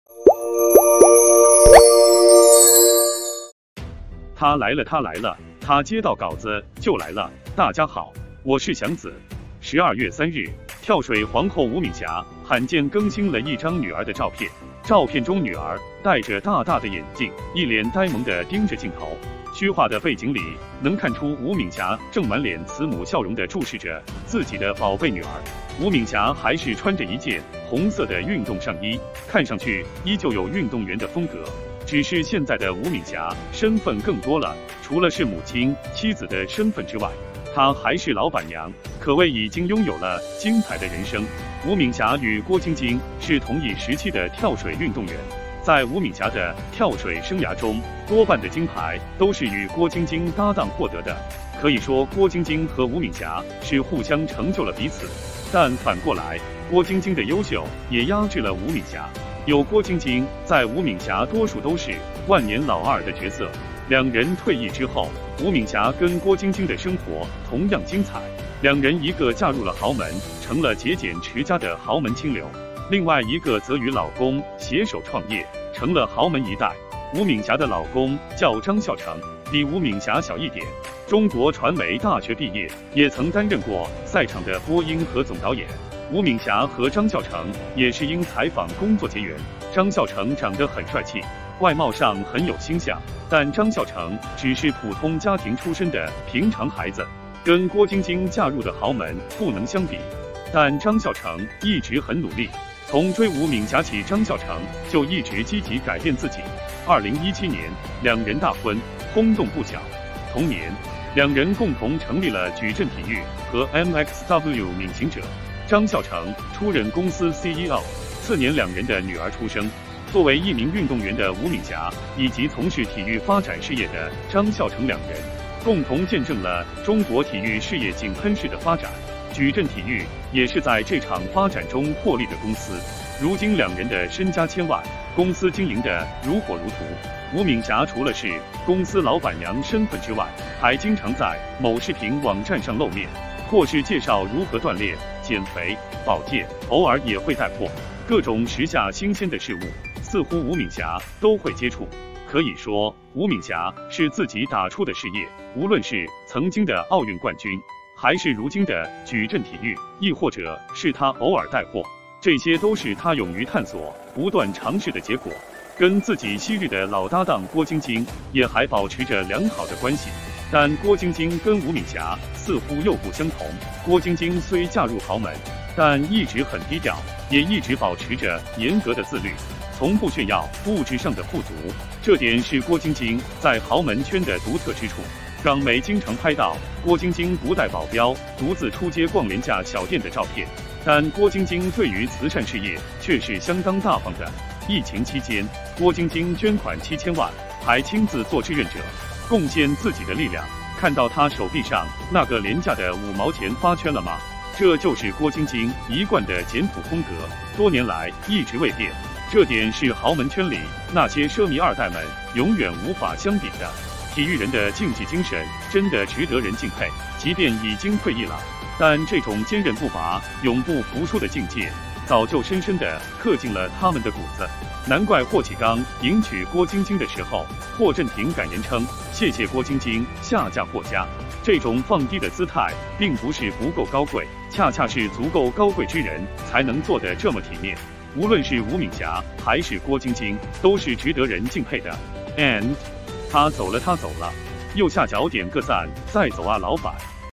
同一位跳水皇后：吴敏霞陪丈夫创业成富一代，郭晶晶成了富豪家庭
12月3日，跳水皇后吴敏霞罕见更新了一张女儿的照片，照片中女儿戴着大大的眼镜，一脸呆萌的盯着镜头，虚化的背景里能看出吴敏霞正满脸慈母笑容的注视着自己的宝贝女儿。